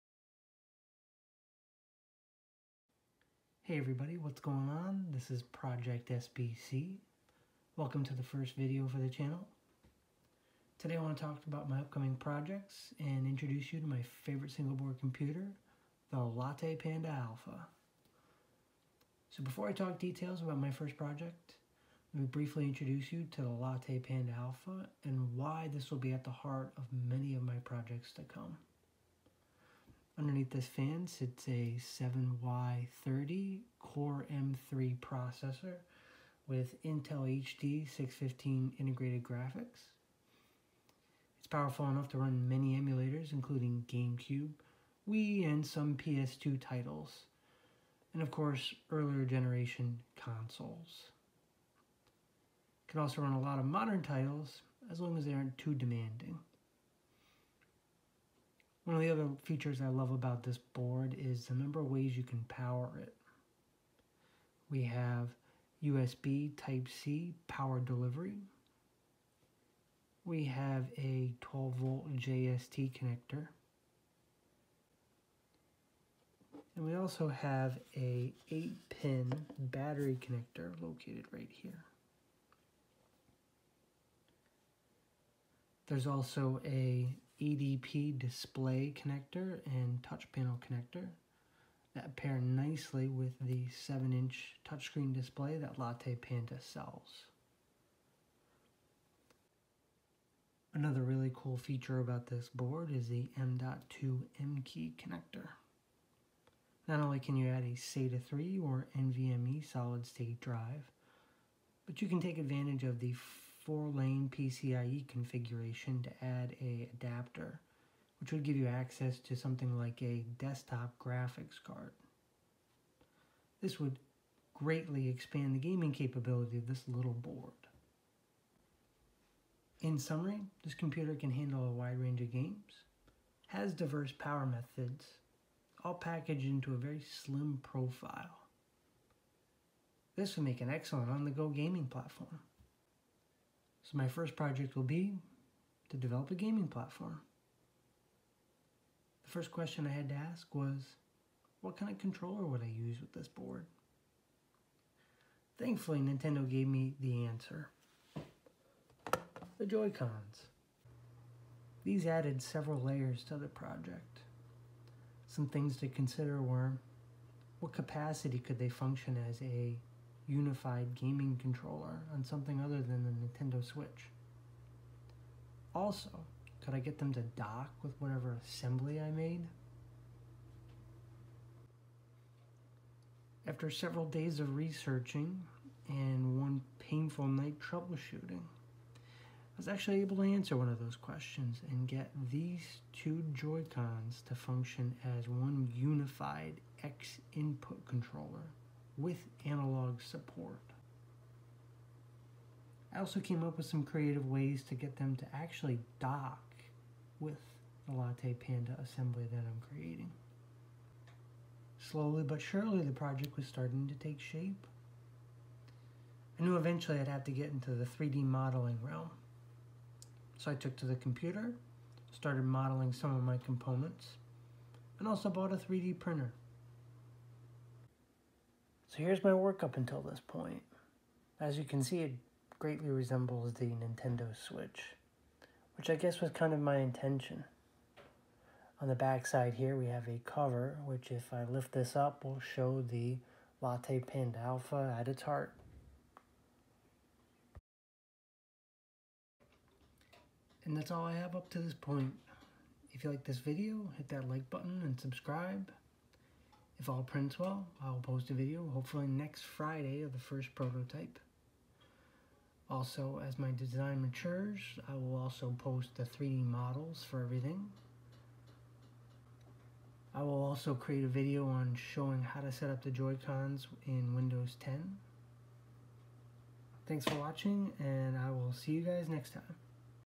Windows 10 Nintendo Switch LattePanda Alpha Project
My first project for this channel; I am designing a on-the-go gaming platform centered around the LattePanda Alpha and using the nintendo switch joycon controllers.

After I print the case and parts, and get it successfully assembled I will upload a new video of the working prototype. Models will be available after the design matures some. I will also make videos on setting up the joycons as xinput controllers.